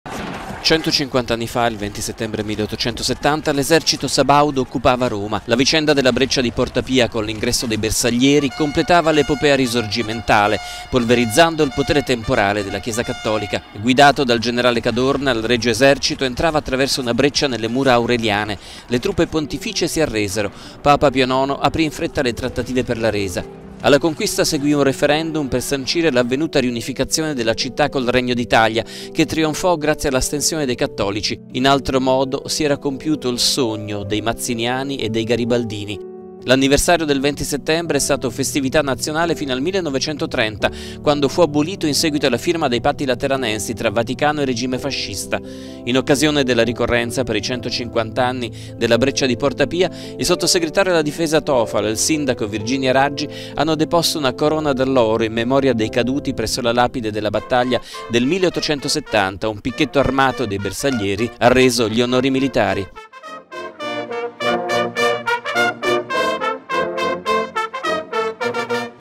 A Porta Pia, 150 anni fa: dalla breccia una nazione
Al culmine del Risorgimento Roma veniva occupata dal Regio Esercito e l'Italia diventava nazione al rango degli altri stati europei e si dotava di una capitale di storica importanza che veniva strappata al papato. Solo il fascismo avrebbe degradato la celebrazione di questa vittoria